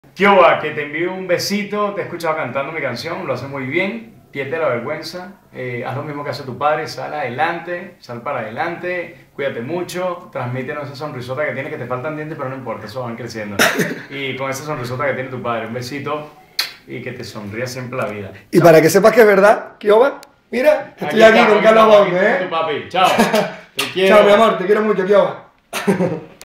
SALUDO DE CARLOS BAUTE A KIOWA, HIJA DE RUDY, CUATRO ELIGEME
UN SALUDO ESPECIAL DE CARLOS BAUTE PARA LA  NIÑA MÁS GUAPA DEL MUNDO.. KIOWA, HIJA DE RUDY